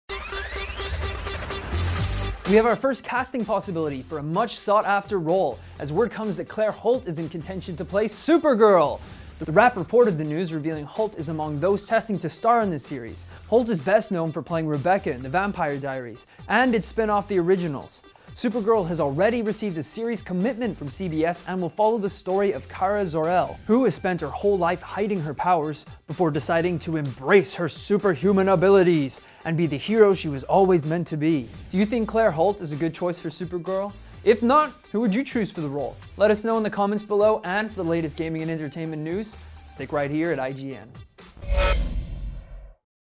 Claire Holt Up For Supergirl TV Show - IGN News
Claire Holt (The Vampire Diaries, The Originals) is among those testing to star in CBS' Supergirl TV series.